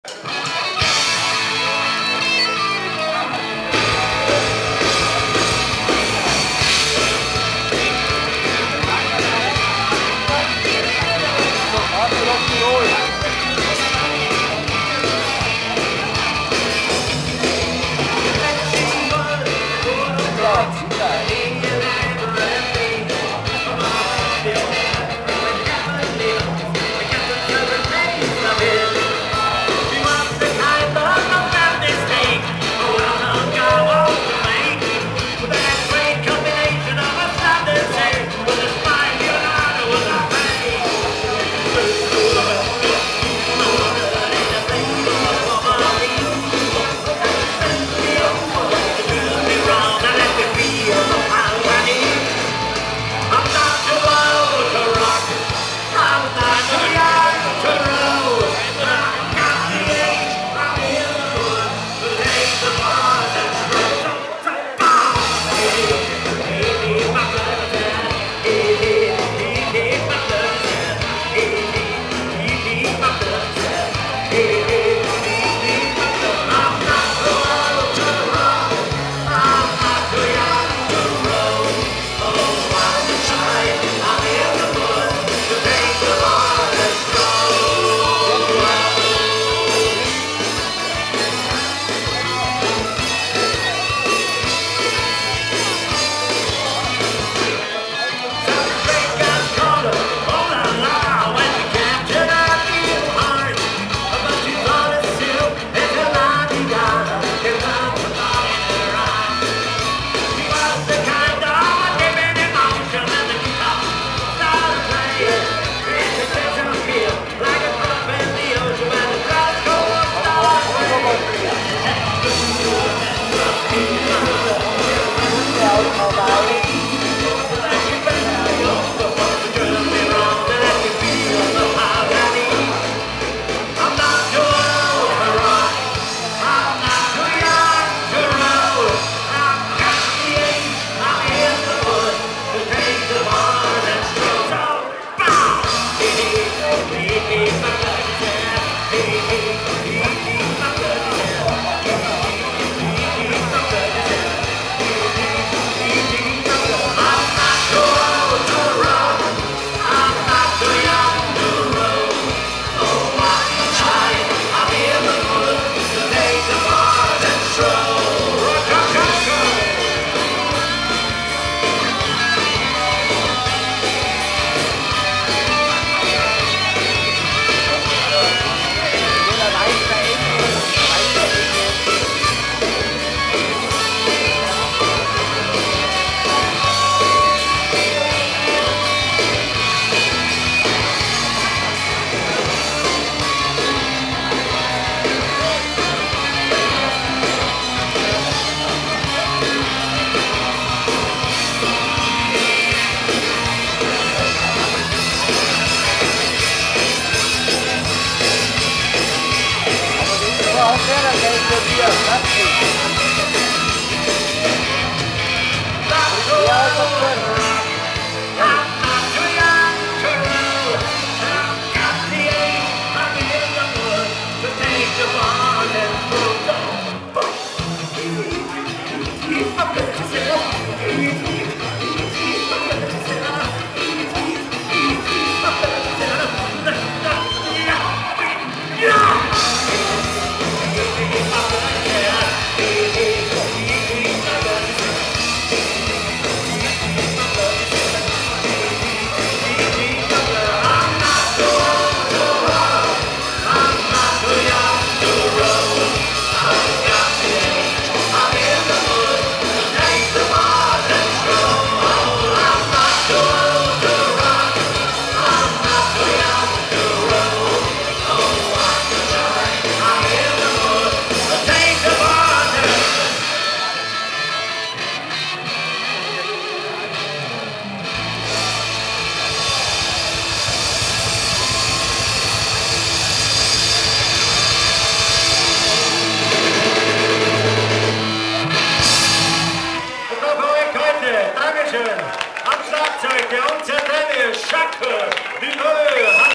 Robespierre & Band in Planegg ( 17.Mai 2009)
Livekonzert am Festplatz Steinkirchen

(Bild-und Tonqualität: -schlecht- Digicam und Gegenlicht)